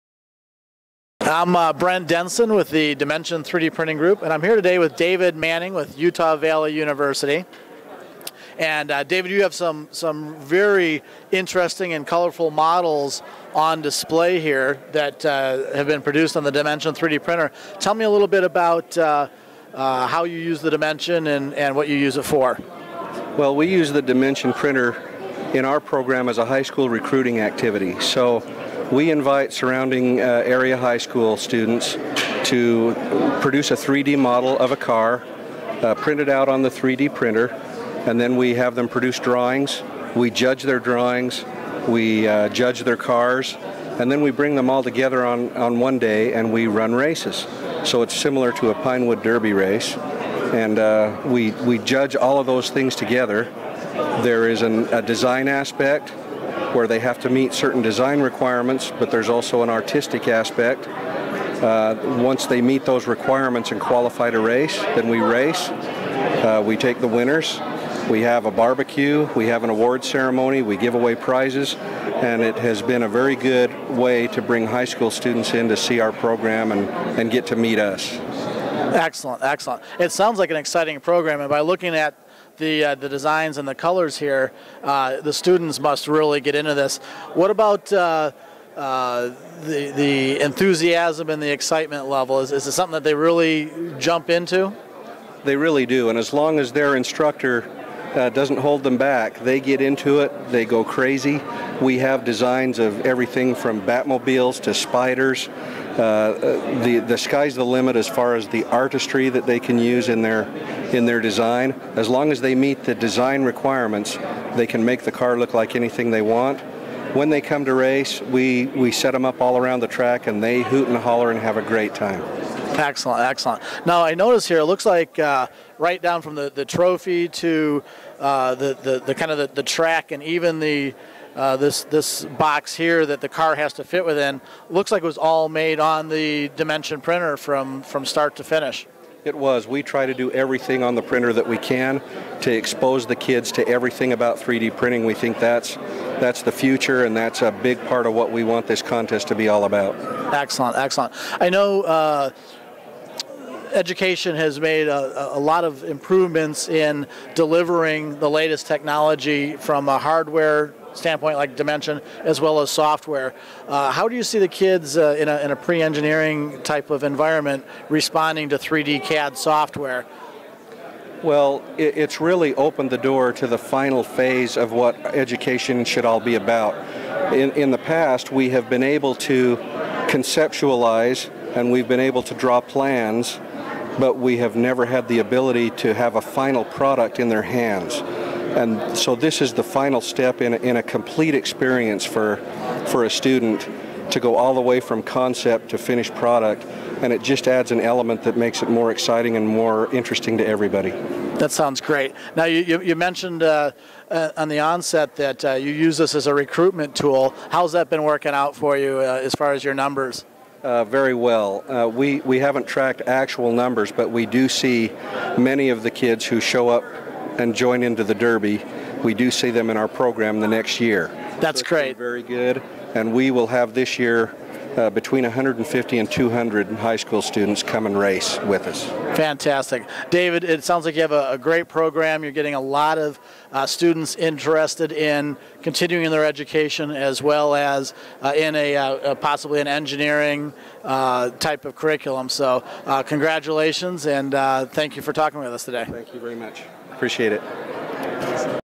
Utah Valley University uses their 3D Printer for a Design Derby Contest
Watch David Manning of Utah Valley University talk about how he uses the Dimension 3D Printer to educate, excite and recruit high school students exploring educational tracks in design and engineering. David discusses the success of Utah Valley Universitys annual pinewood derby-like design contest and race for high school students. for more information on Utah Valley University.